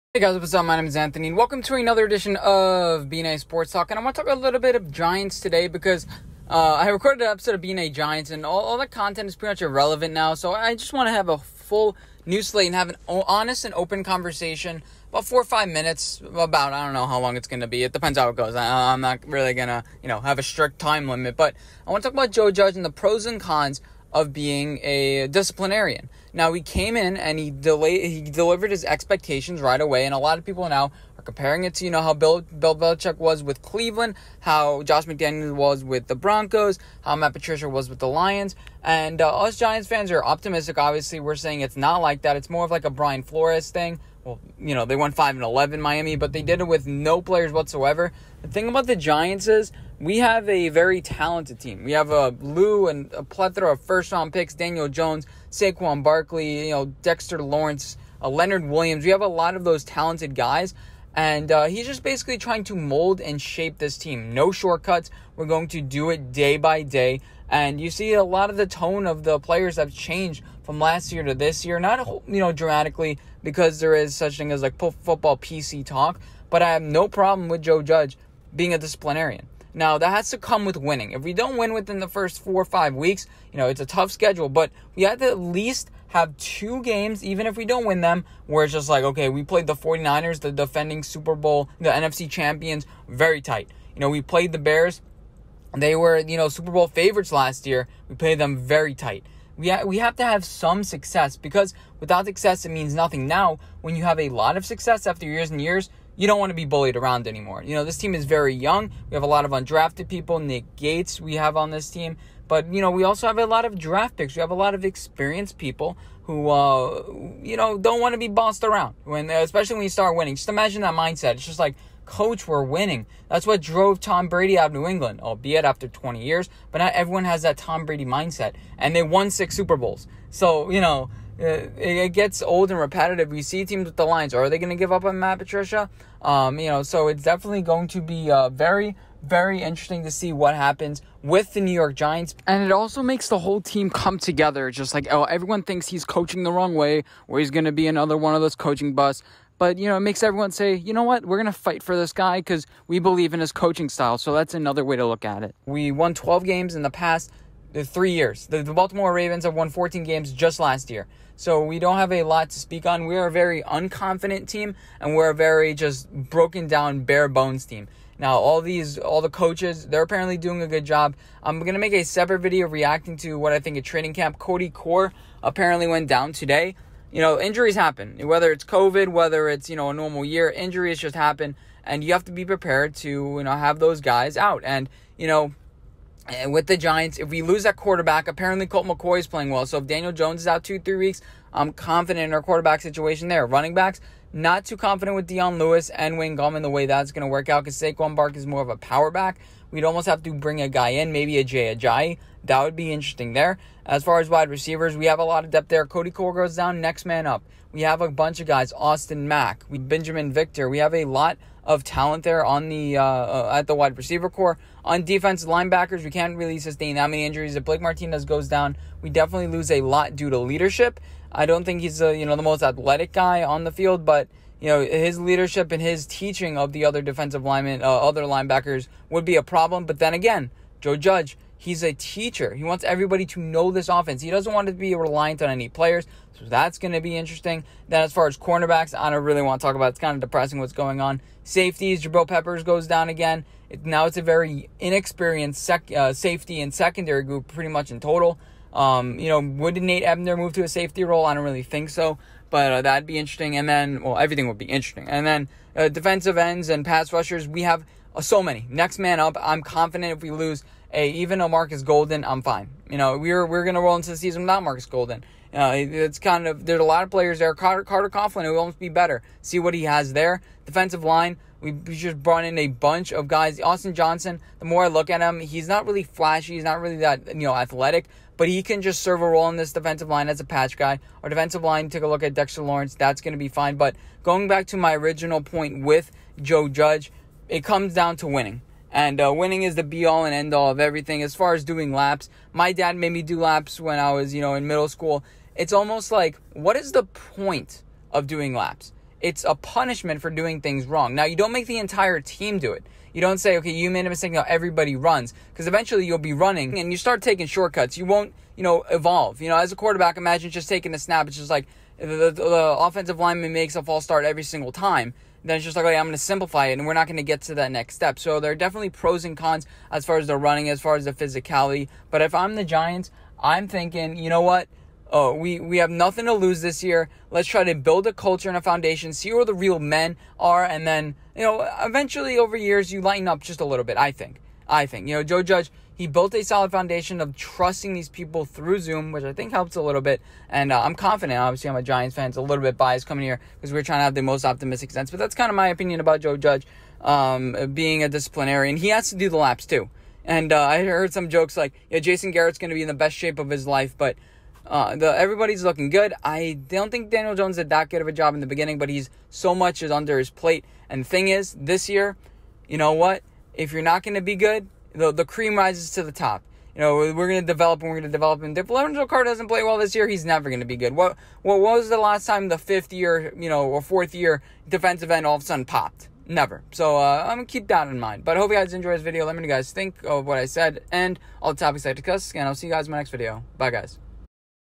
Is JUDGE too Harsh of a Coach? From a GIANTS FAN (309)
There has been speculation that Judge might be too hard on players. I give you my take. As always, let me know your opinion on any and all other topics we discussed in this video below. Enjoy the video!